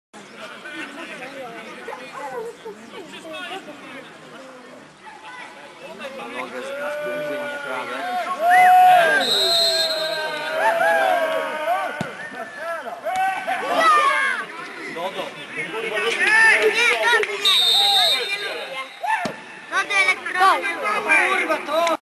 Roztocze Narol 2-0 Golbalux Wiązownica [golbaluxwiazownica.pl]
rzuty wolne Tomka Babiarza i Mariusza Orzechowskiego